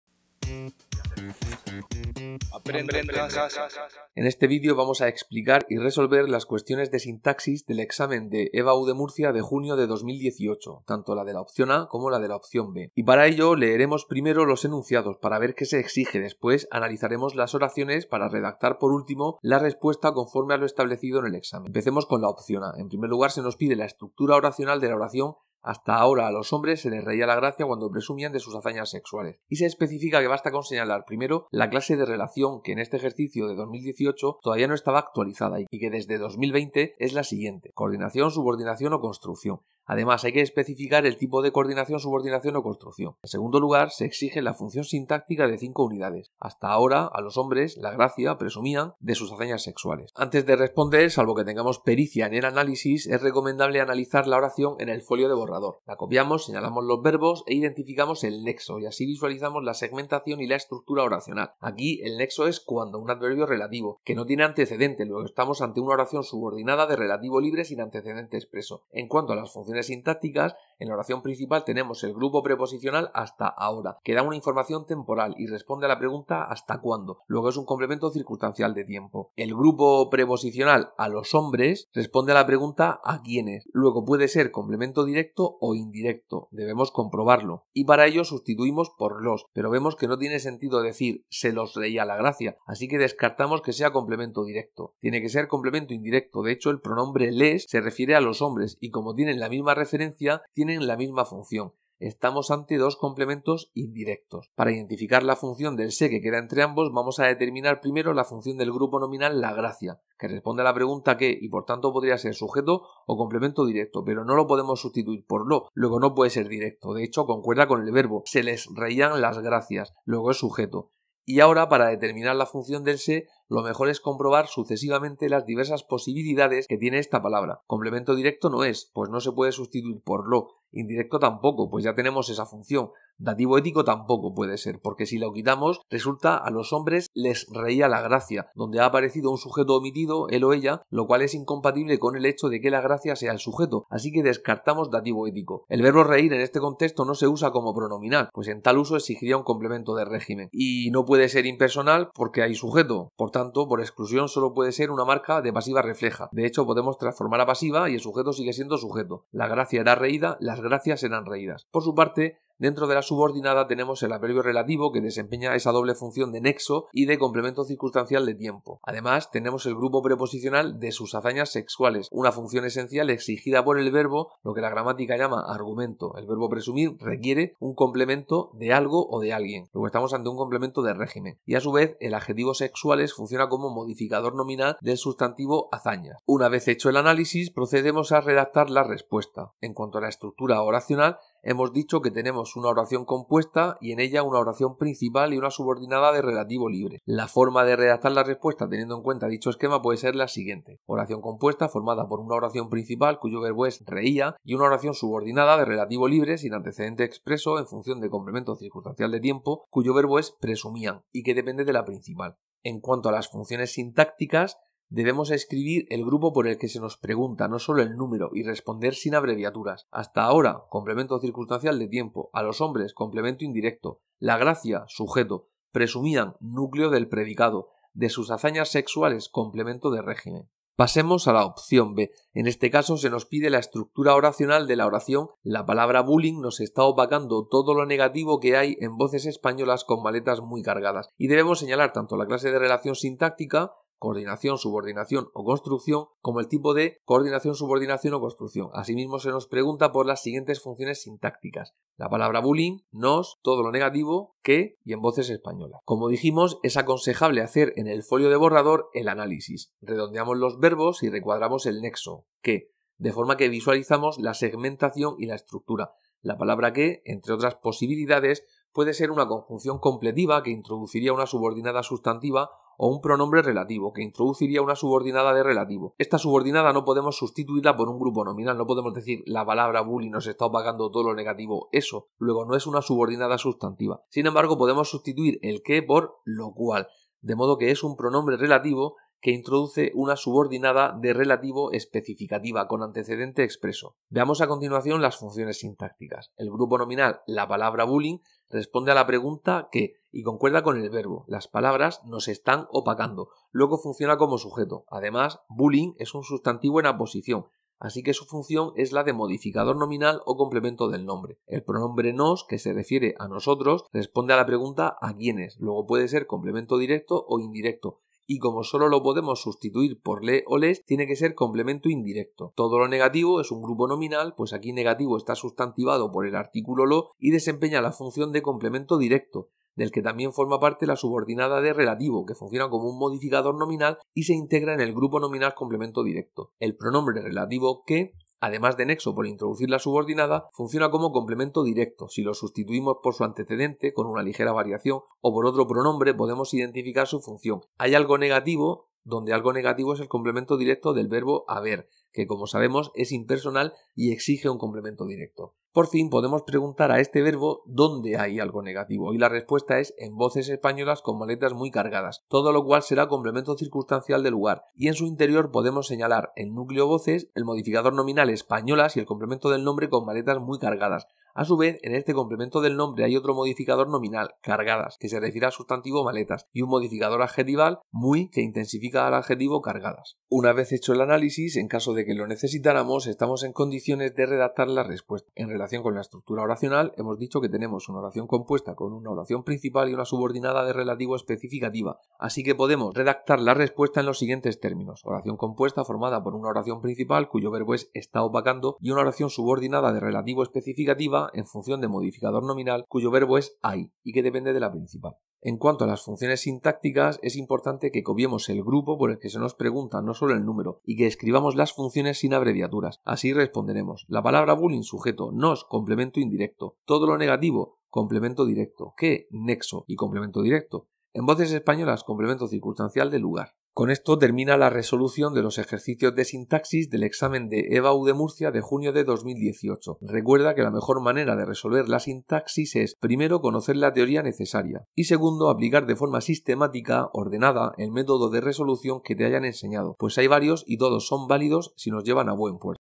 SINTAXIS EBAU JUNIO 2018
Resolución de los ejercicios de sintaxis del examen de Lengua Castellana y Literatura de EBAU de Murcia de junio de 2018.